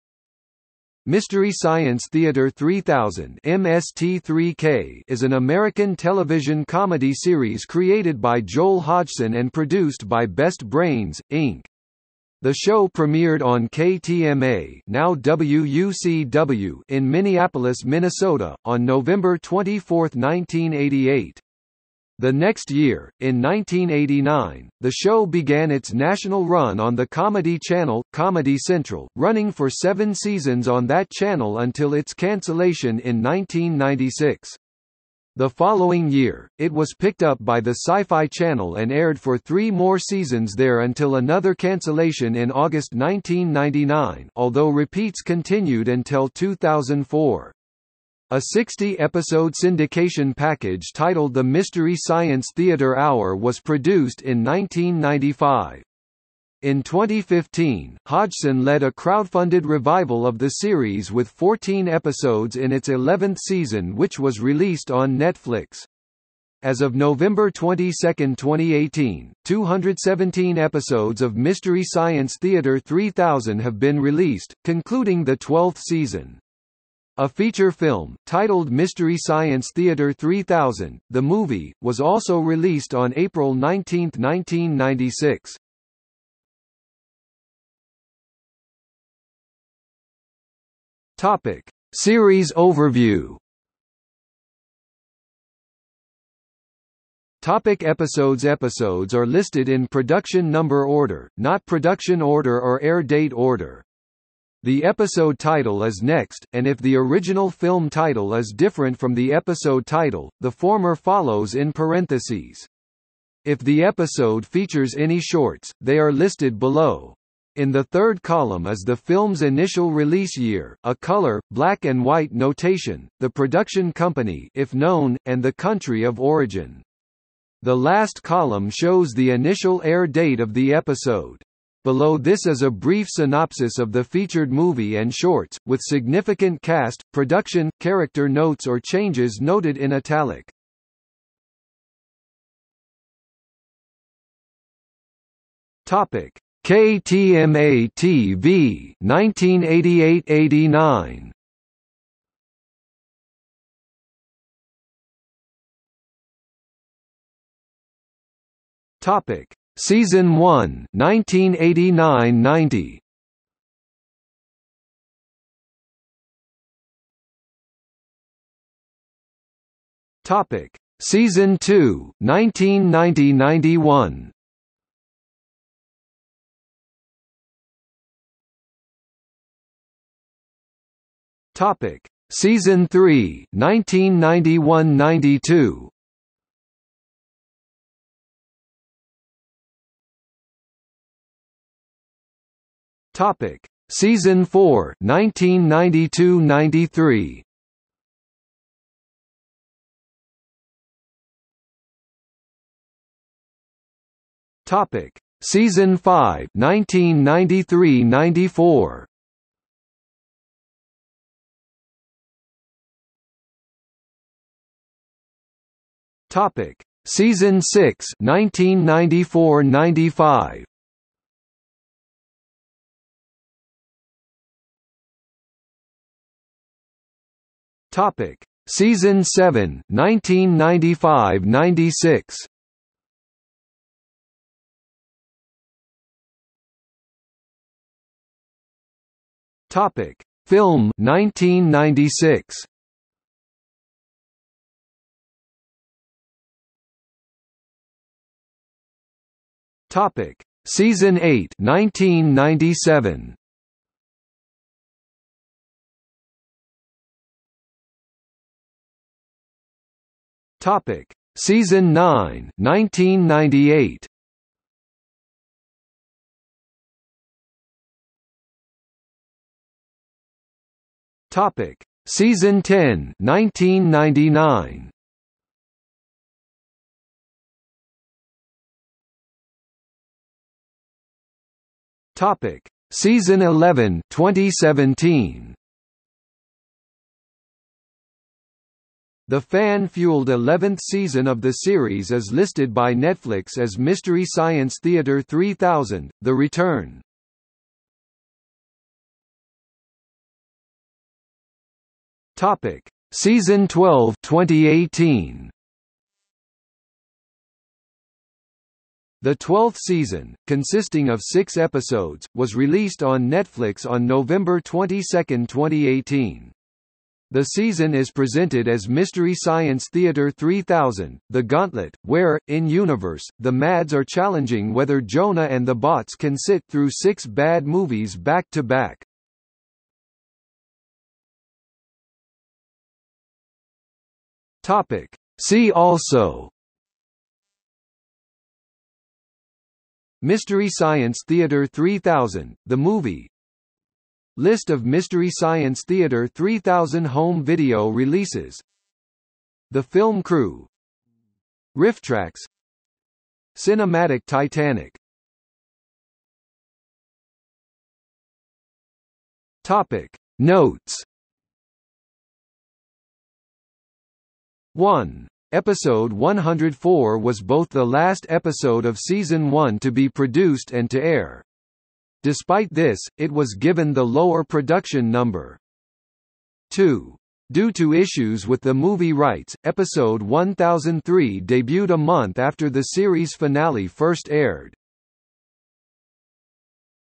Mystery Science Theater 3000 (season 10) | Wikipedia audio article
00:01:27 1 Series overview
00:01:37 2 Episodes
00:02:22 2.1 KTMA-TV (1988–89)
00:02:35 2.2 Season 1 (1989–90)
00:02:46 2.3 Season 2 (1990–91)
00:02:58 2.4 Season 3 (1991–92)
00:03:09 2.5 Season 4 (1992–93)
00:03:21 2.6 Season 5 (1993–94)
00:03:32 2.7 Season 6 (1994–95)
00:03:44 2.8 Season 7 (1995–96)
00:03:56 2.9 Film (1996)
00:04:06 2.10 Season 8 (1997)
00:04:17 2.11 Season 9 (1998)
00:04:27 2.12 Season 10 (1999)
00:04:38 2.13 Season 11 (2017)
00:04:59 2.14 Season 12 (2018)
00:05:31 3 See also
00:05:56 4 Notes

- Socrates

SUMMARY
Mystery Science Theater 3000 (MST3K) is an American television comedy series created by Joel Hodgson and produced by Best Brains, Inc. The show premiered on KTMA (now WUCW) in Minneapolis, Minnesota, on November 24, 1988. The next year, in 1989, the show began its national run on The Comedy Channel/Comedy Central, running for seven seasons on that channel until its cancellation in 1996. The following year, it was picked up by The Sci-Fi Channel and aired for three more seasons there until another cancellation in August 1999 (although repeats continued until 2004). A sixty-episode syndication package titled The Mystery Science Theater Hour was produced in 1995. In 2015, Hodgson led a crowdfunded revival of the series with 14 episodes in its eleventh season which was released on Netflix. As of November 22, 2018, 217 episodes of Mystery Science Theater 3000 have been released, concluding the twelfth season. A feature film, titled Mystery Science Theater 3000: The Movie, was also released on April 19, 1996.